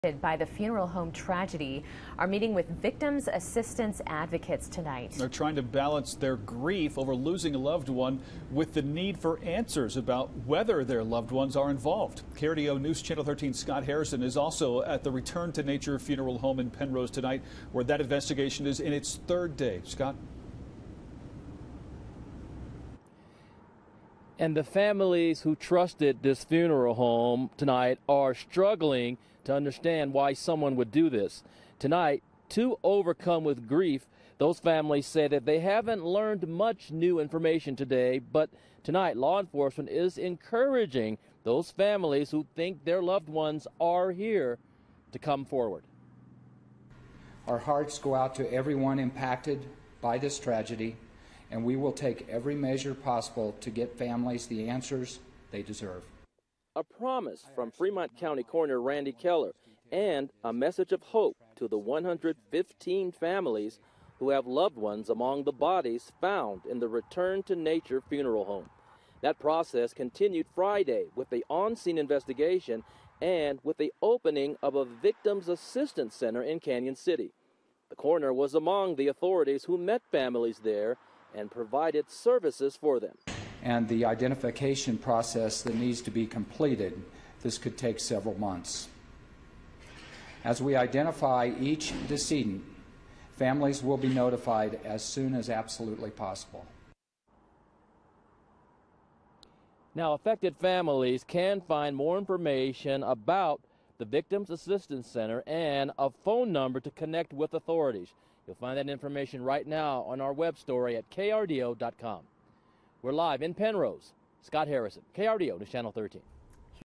Families who used Return to Nature Funeral Home asked to please come forward
Families affected by the investigation into a Penrose funeral home for the improper storage of bodies are asked to contact law enforcement as soon as possible.  | Read the full story